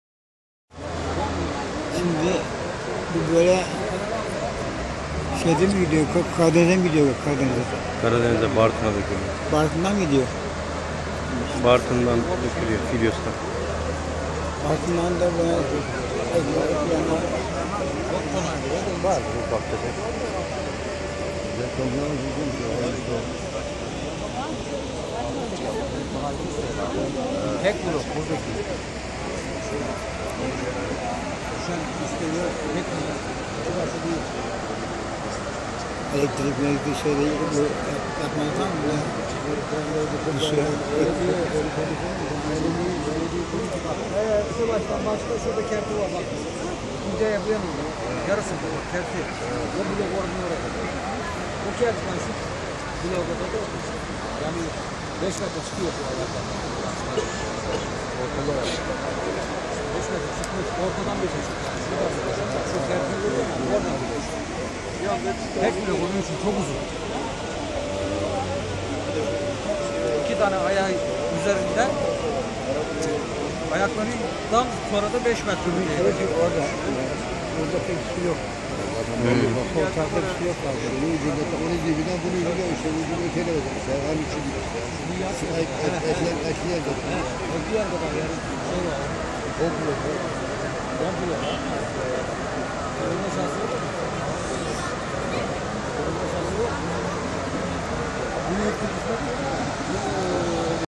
Zonguldak Çaycuma köprüsünün yıkılması
ZONGULDAK’ın Çaycuma İlçesi’nde, 6 Nisan 2012’de 15 kişinin öldüğü köprü